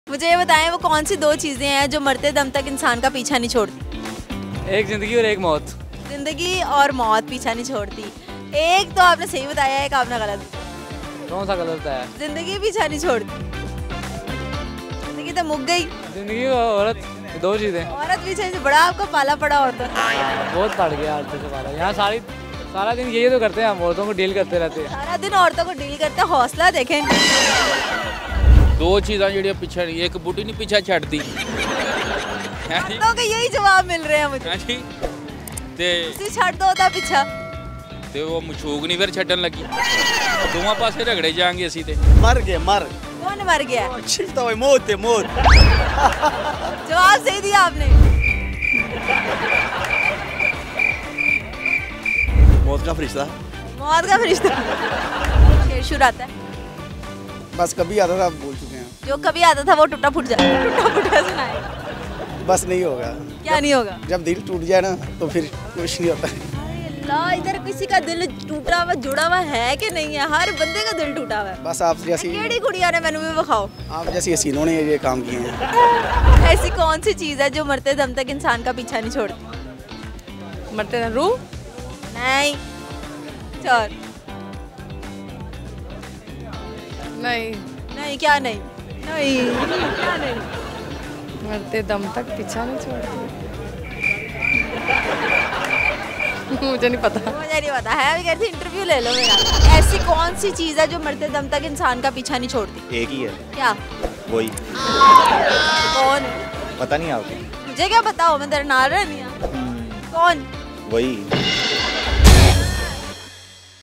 Funny Questions with Public | Mic Drop Roadshow | Episode 30
دو چیزیں جو مرتے دم تک انسان کا ساتھ نہیں چھوڑتی؟؟